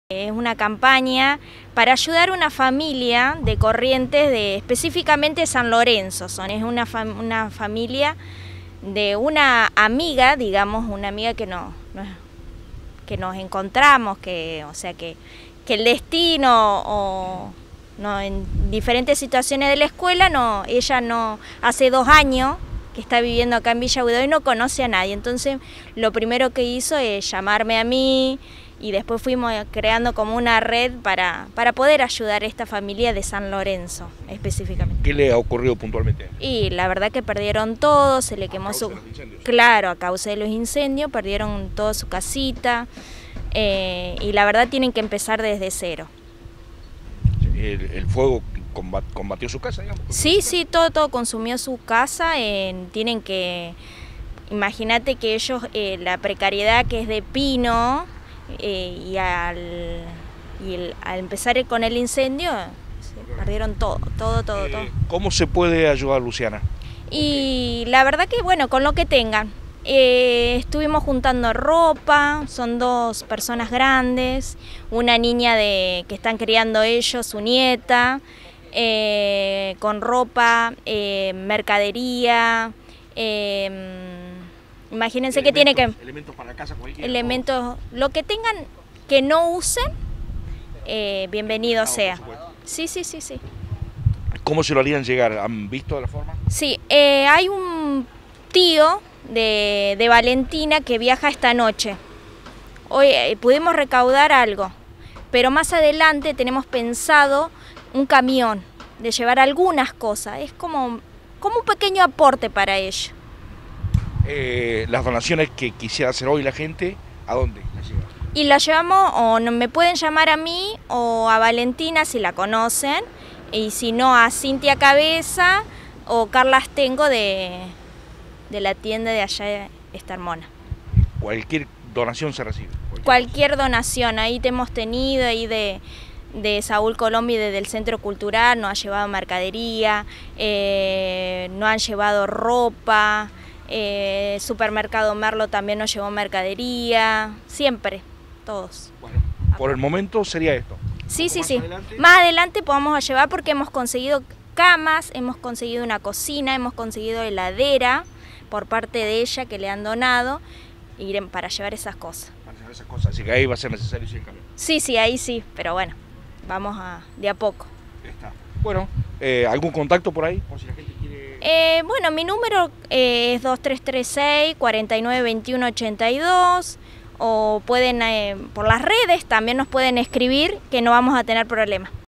COLECTA SOLIDARIA PARA UNA FAMILIA DE CORRIENTES QUE LO PERDIÓ TODO
Los incendios en la Provincia de Corrientes han dejado a muchas familias sin absolutamente nada, es el caso de una familia de la localidad de San Lorenzo, donde las llamas alcanzaron su vivienda perdiendo todo lo que tenían, desde nuestra localidad se ha organizado una colecta para poder colaborar con esta difícil situación, compartimos la palabra de una de las encargadas Lucia Colombi, que nos brinda los detalles.